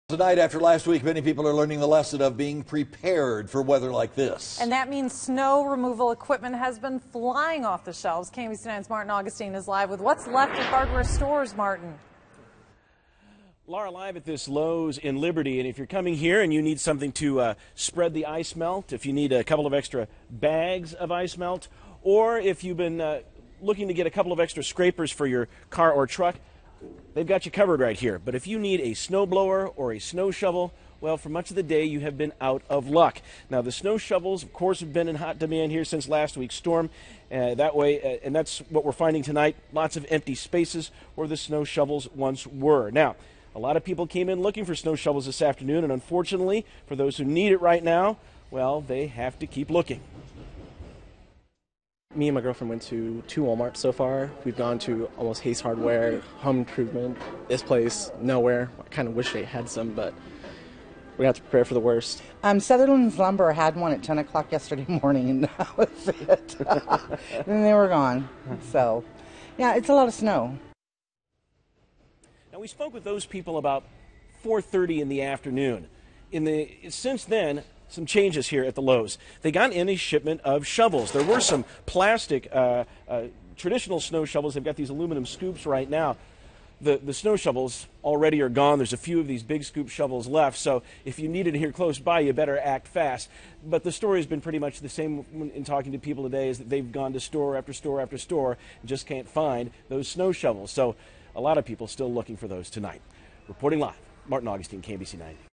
Stores see run on snow-removal supplies
Hardware and home improvement stores said they've had a lot of customers coming in over the past week looking for snow shovels, ice melt and other things to help get rid of snow.